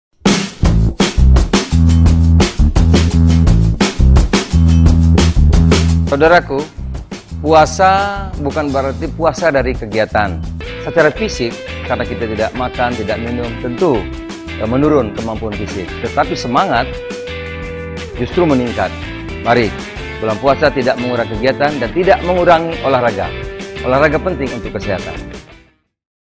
Puasa tidak menghalangi untuk berolahraga
Dokumentasi
Ketum PAN tetap berolahraga walaupun sedang berpuasa